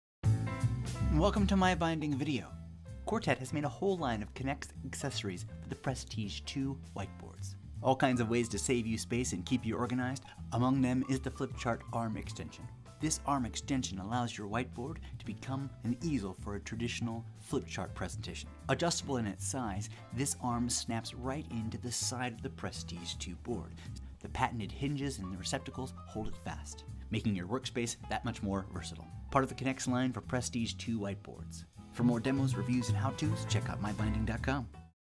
Quartet Prestige 2 Connects Flipchart Extension Arm Demo
You can purchase and compare prices of this and many other products on out site at:

For more writable space, this Flip-chart Extension Arm allows a flip chart to hang directly next to your whiteboard instead of over the top. You get the additional convenience of the flip-chart pages without sacrificing any of the dry-erase writing surface. The retractable panel slides out from behind the whiteboard to the side. Flip charts easily attach to the arm. Panel conveniently retracts when not in use. Extension arm must be used in conjunction with a Quartet Prestige 2 whiteboard (sold separately). Combine with other Prestige Connects Accessories for a fully functional, convenient and collaborative work space.

For details on this product and many more go to MyBinding.com!